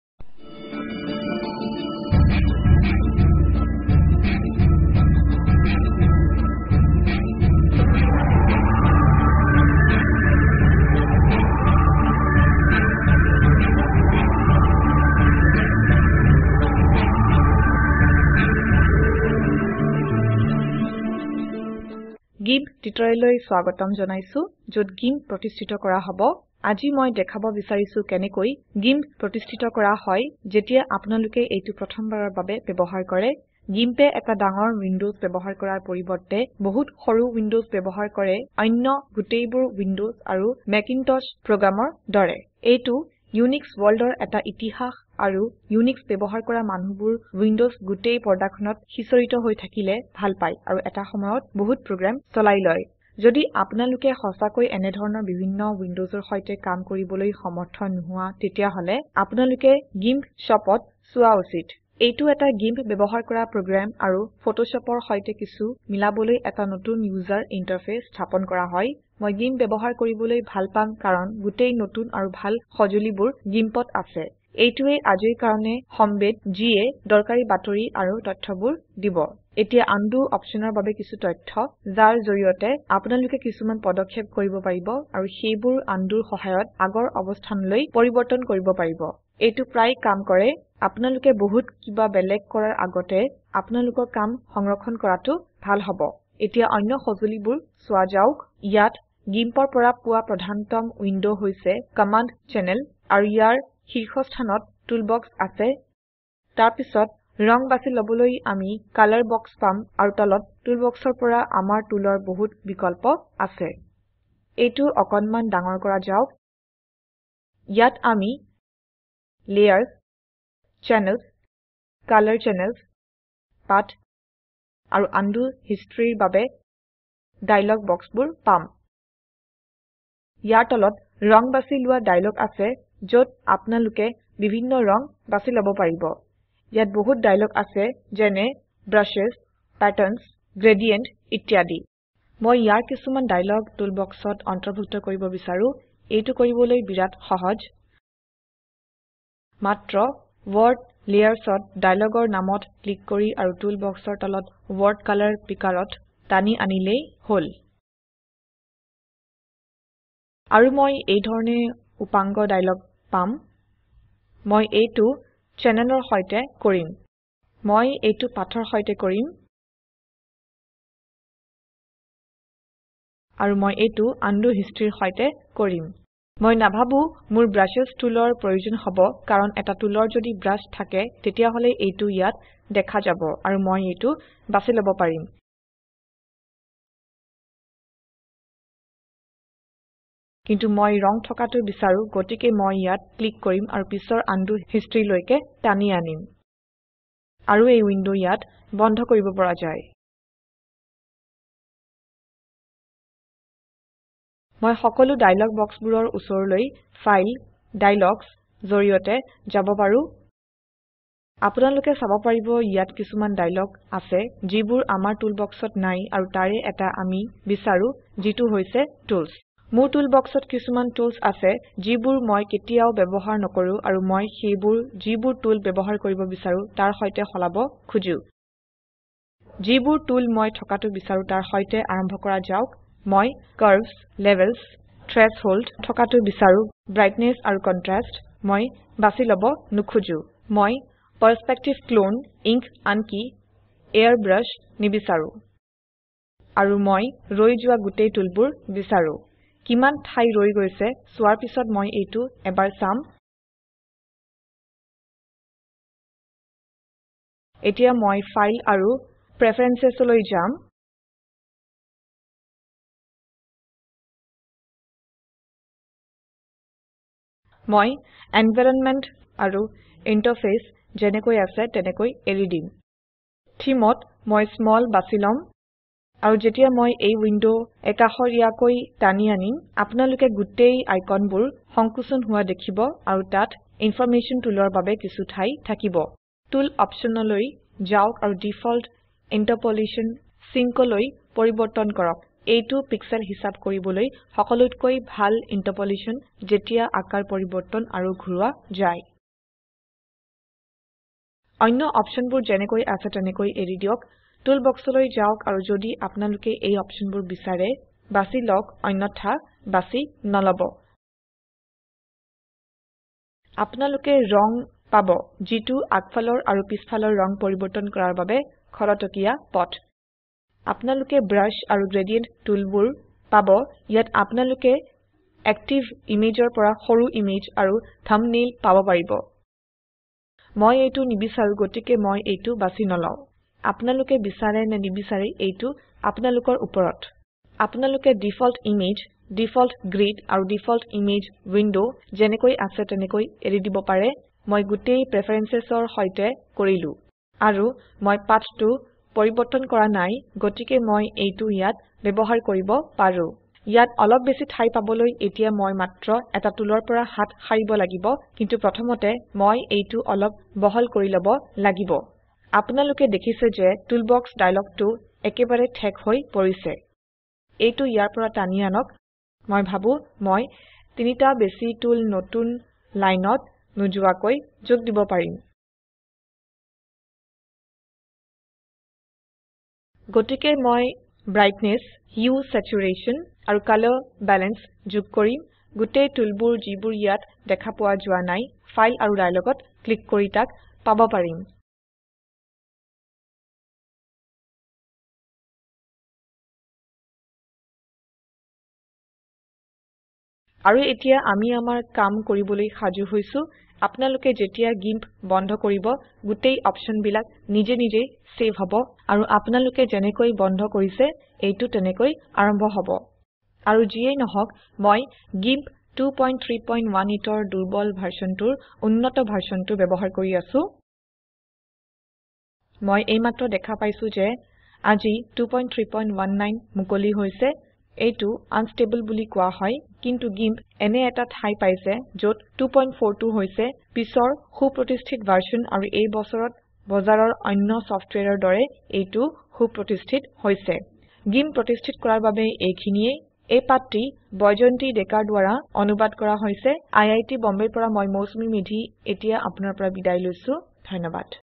Setting Up GIMP - Assamese
প্রথমবাৰৰ ব্যবহাৰকাৰীৰ বাবে GIMP সেট আপ কৰা GIMPৰ প্রধান উইন্ডো - কমান্ড কেন্দ্রীয়(Central) GIMP ইন্টাৰফেসত বিভিন্ন সৰু ৱিন্দোৰ ব্যাখ্যা: (1) টুলবক্স, (2) কালাৰ বাক্স, (3) লেয়াৰ্চ বক্স, (4) চ্যানেল বক্স, (5) কালাৰ চ্যানেল বক্স, (6) পাথ বক্স, (7) হিস্ট্ৰী বক্স পূর্বাবস্থান কৰা